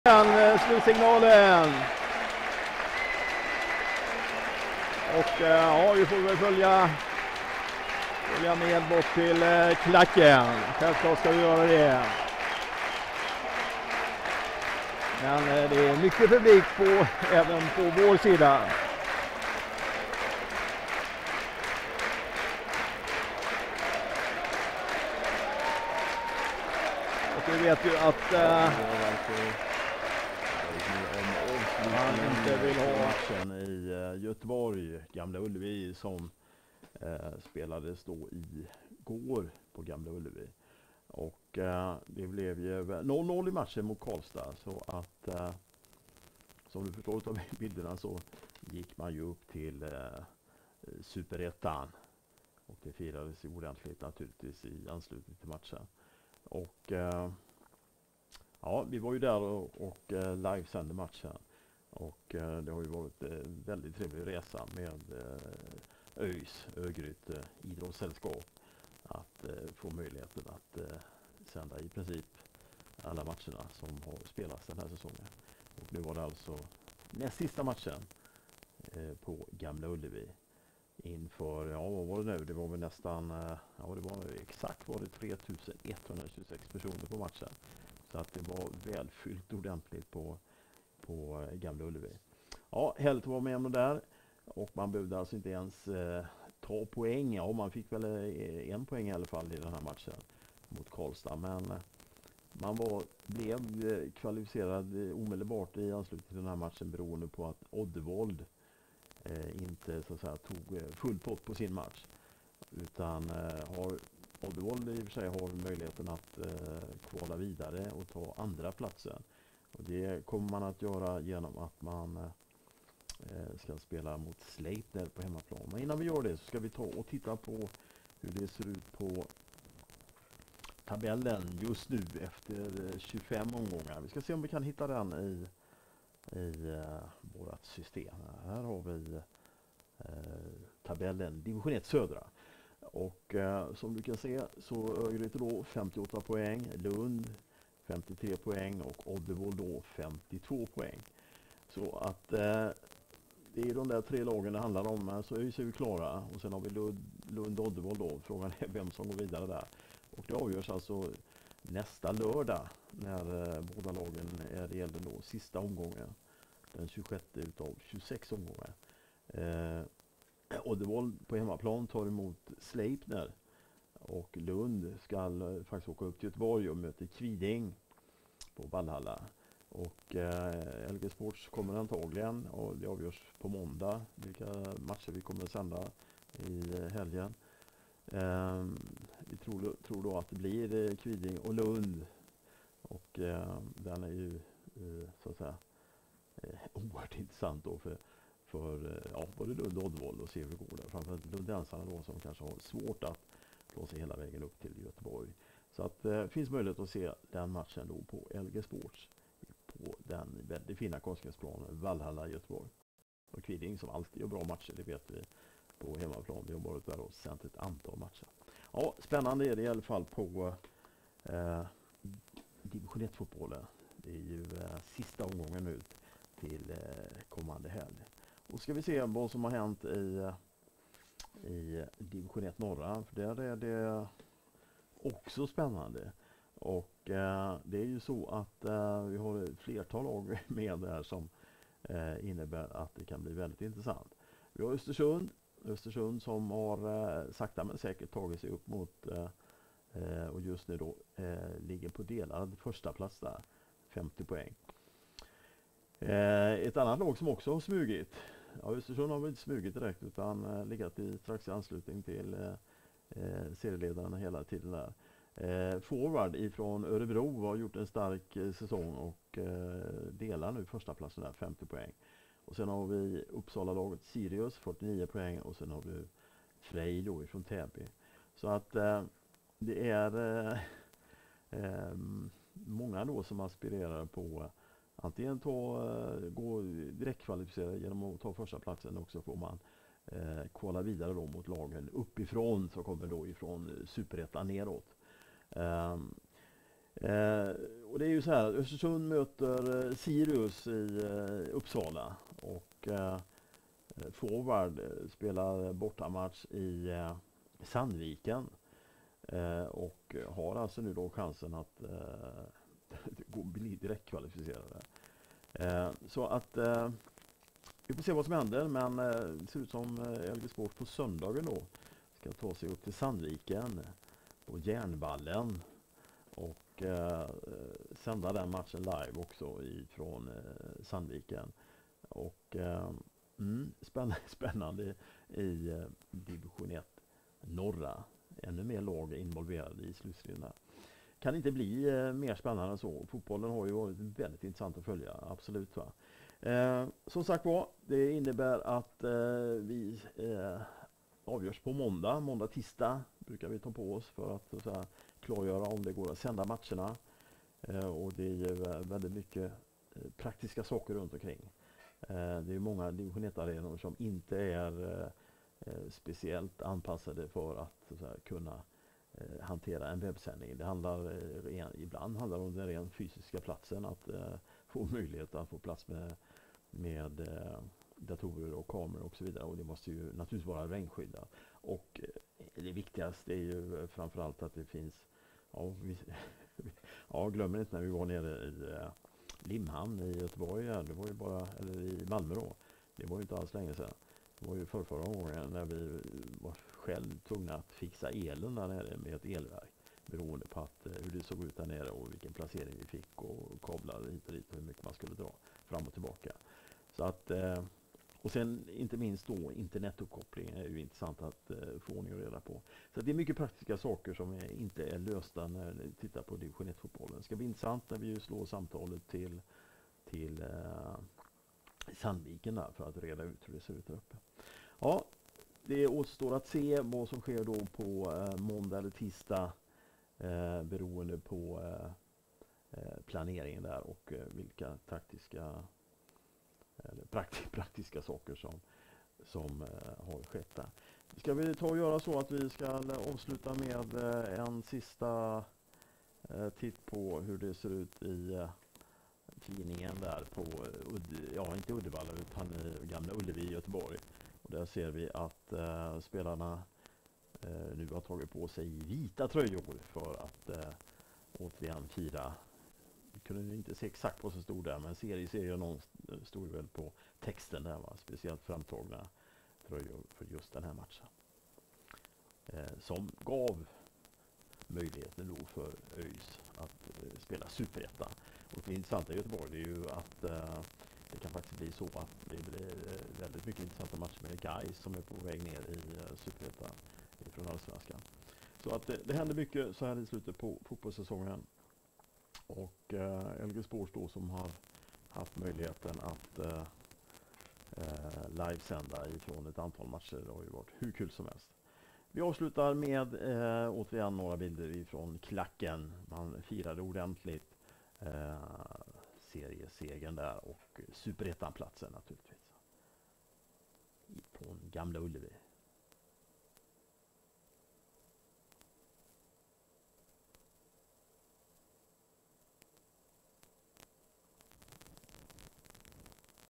LGSTUDIO Fotboll 121021
LGSTUDIO funderar över Div 1 Fotbollens sista matcher som avgör vilka lag som går till SuperEttan.